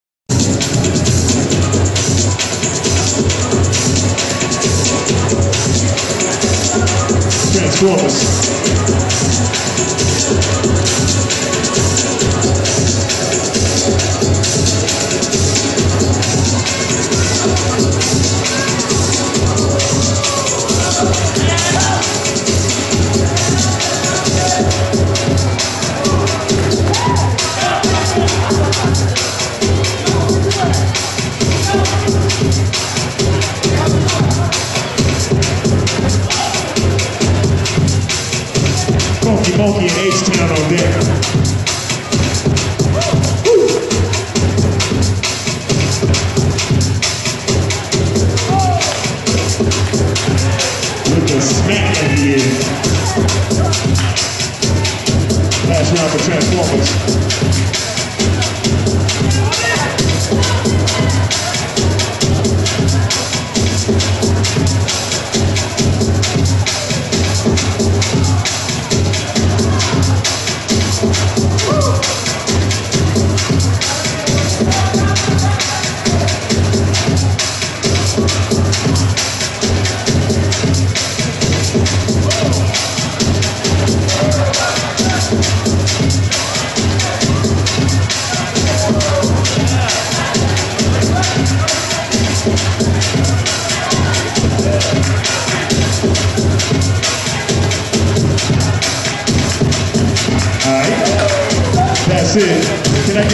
Rhythm &Souls2.wmv
Bboy Prophet(TWS) and Bboy Blue vs. Bboy Limestone and Bboy Lizard(Tranzformerz) El Paso Tx.